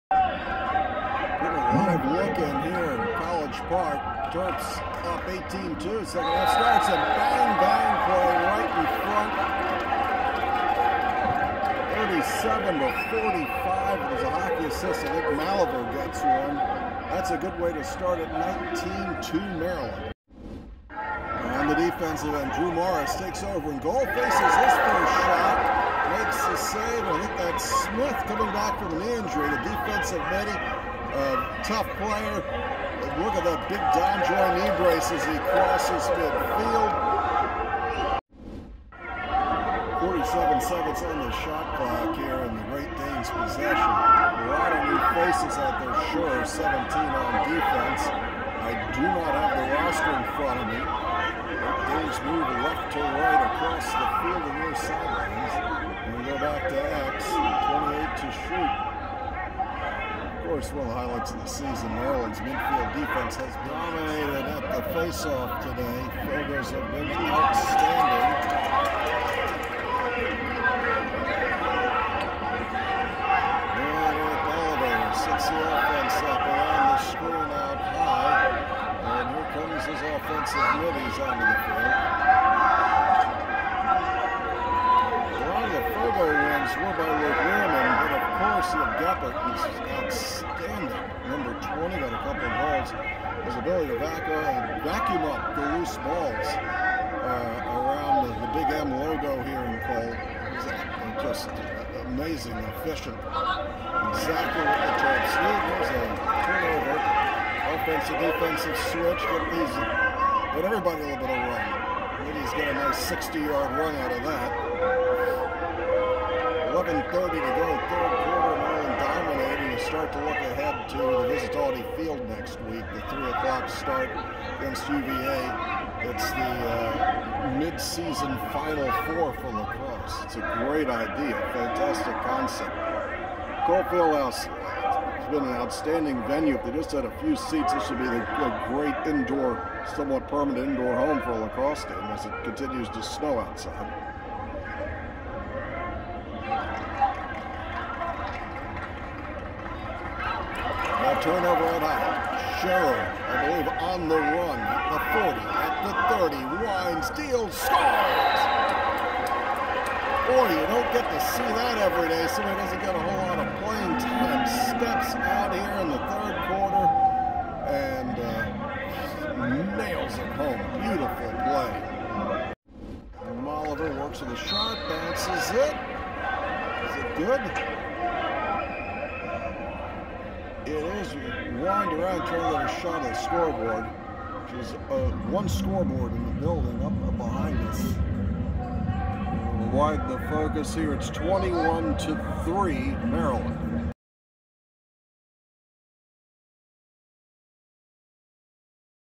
Maryland lacrosse 2022 - Onfield pxp Terps 24 Albany 6
Here are excerpts from the 2nd half of the Maryland win.  Includes the Sherrer goal and 1st save by Drew Morris.  You get to see many of the guys who don't regularly play in games from a point blank, on field view.

Wayne Viener on play by play. Shot on Samsung equipment in 4K resolution.